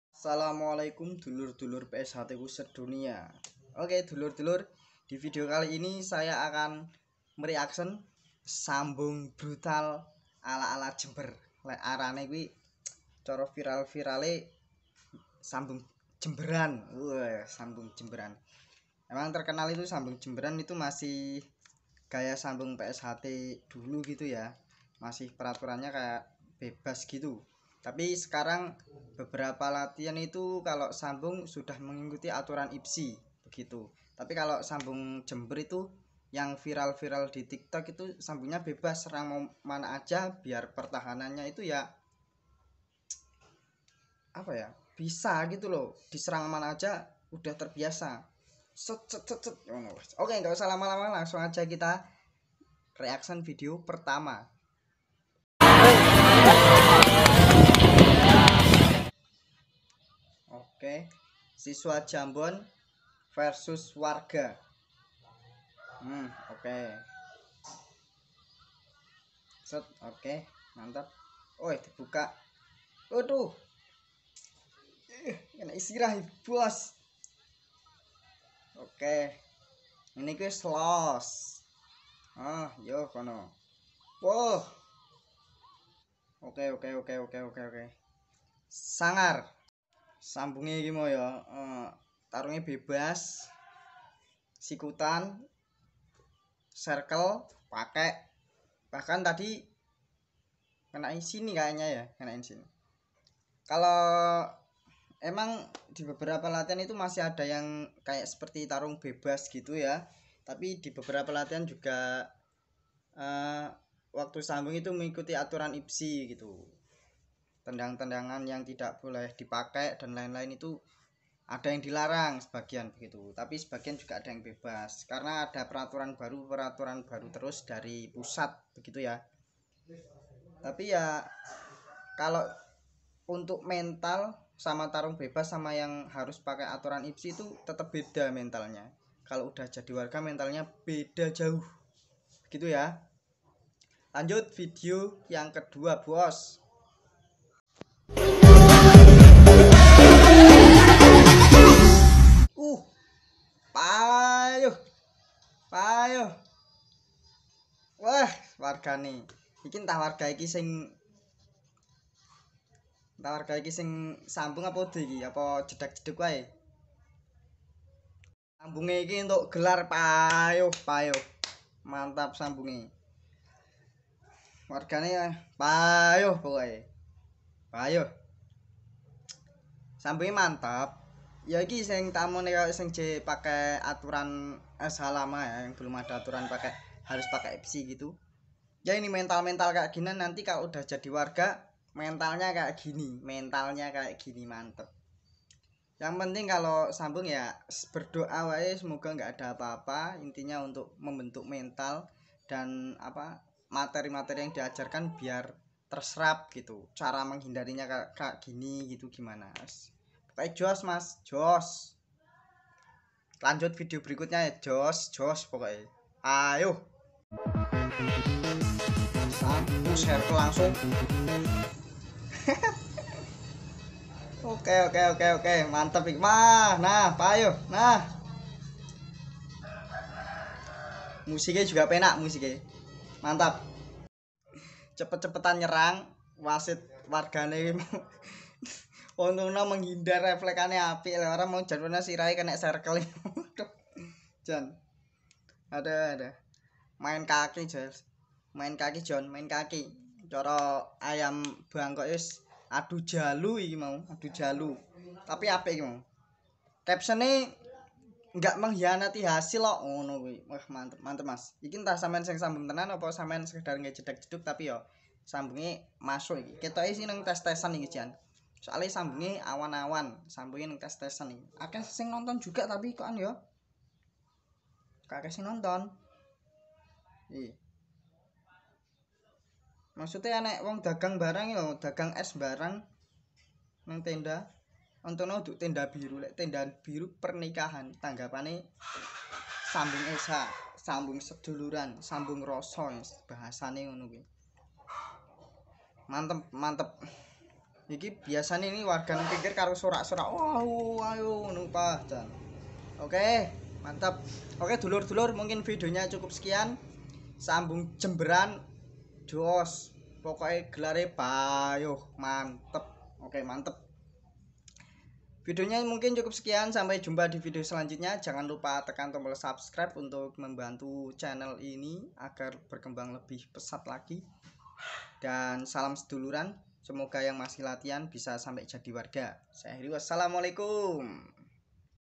sambung ala jember psht emang joSHT tidak menghianati hasil peraturan lawas psht
sambung ala jember masih menggunakan peraturan lama psht